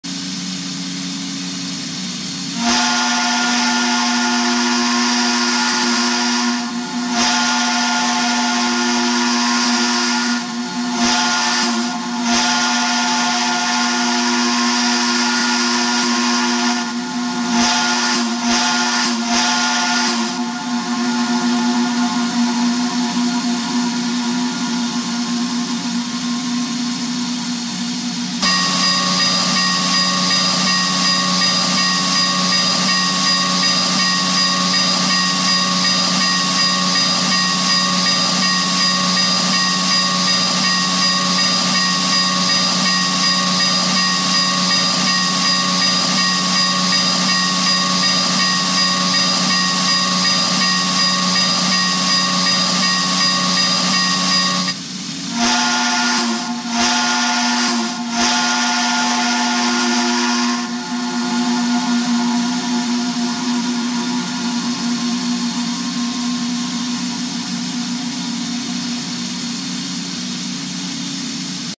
SSW 819 Frisco 3 Chime V1(3 Part)
Here's Cotton Belt 4-8-4 L-1 Northern #819's Frisco 3 Chime Whistle V1 (3 Part).